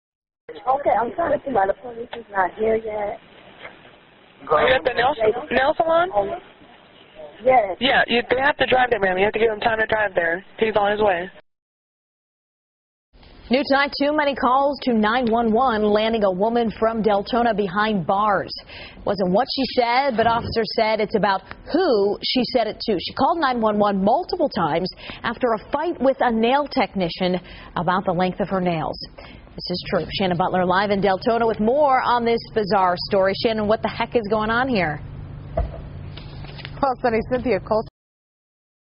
Lady Arrested For Calling 911 After Nail Shop
Fk'd Up Her Nails!